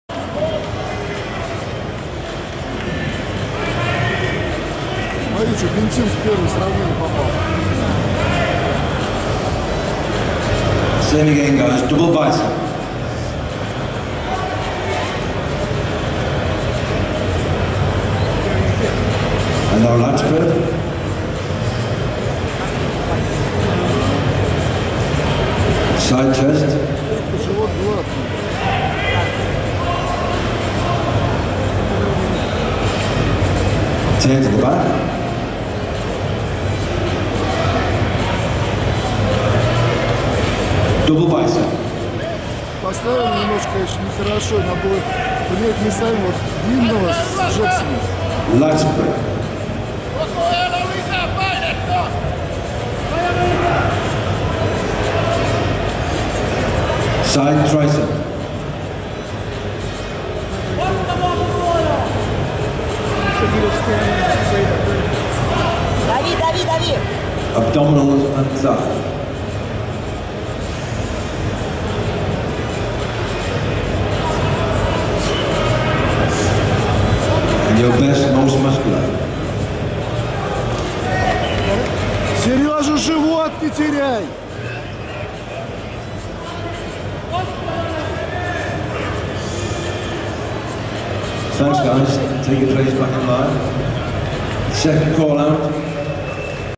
Sergey Shelestov in Call-Out @ FIBO 2012 (2)
Sergey Shelestov (Сергей Шелестов) at a call-out @ the IFBB 2012 FIBO Pro. From left to right: no. 1 Steve Benthin (Germany; 7th), no. 5 Sergey Shelestov (Russia; 3rd), no. 6 Mike Kefalianos (Australia; 2nd), no. 8 Saied Feras (Syria, 5th) and no. 11 Johnnie Jackson (USA; 1st).